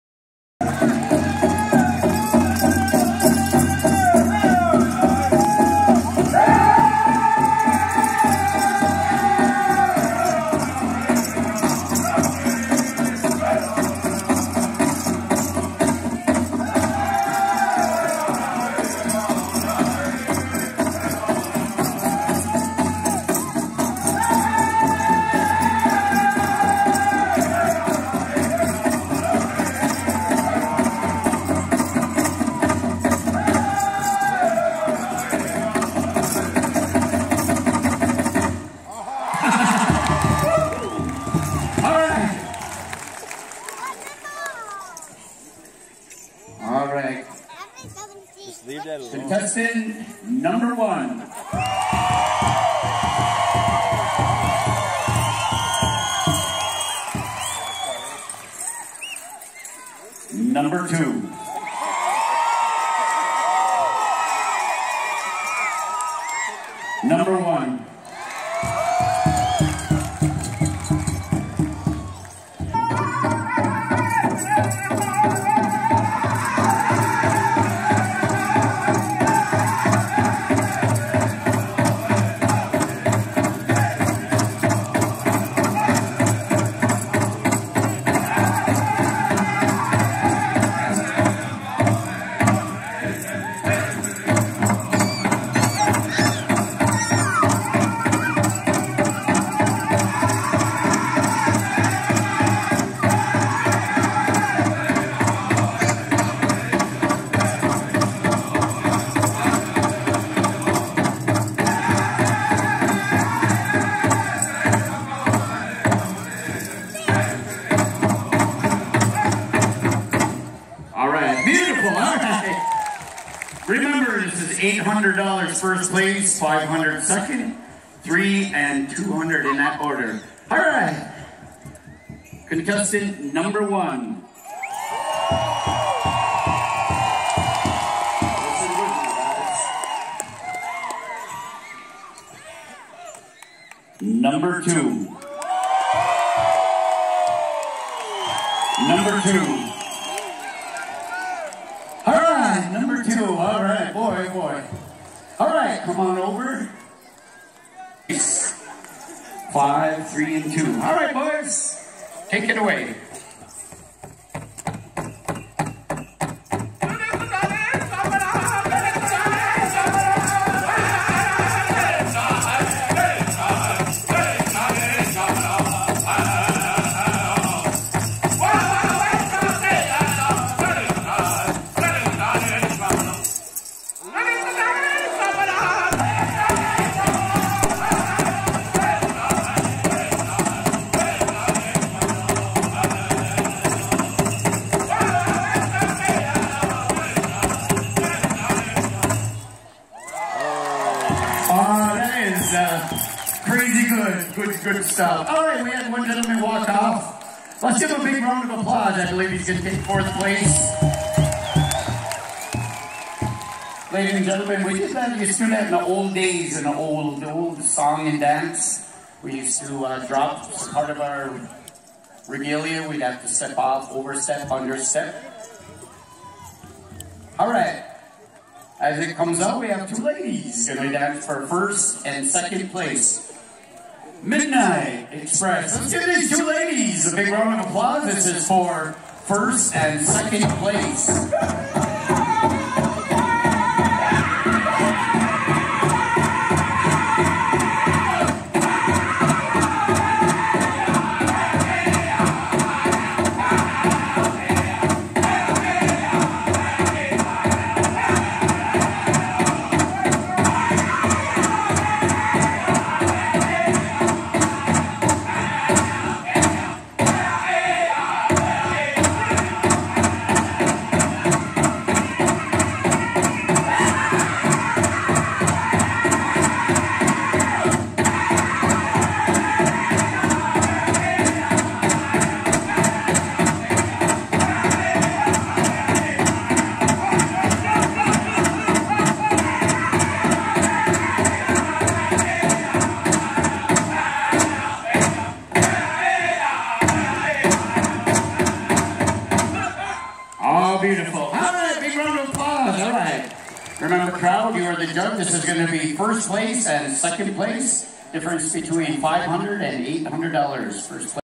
Fancy vs fancy @ Menominee Nation contest Pw 2016
Finals. In the special. Saturday night main event